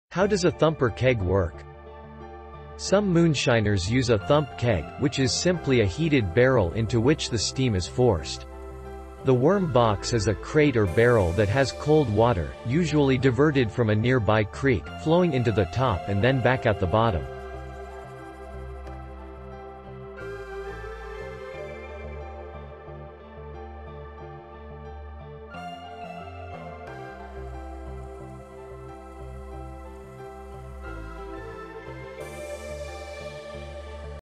How does a thumper keg work?
Some moonshiners use a thump keg, which is simply a heated barrel into which the steam is forced. ... The worm box is a crate or barrel that has cold water, usually diverted from a nearby creek, flowing into the top and then back out the bottom.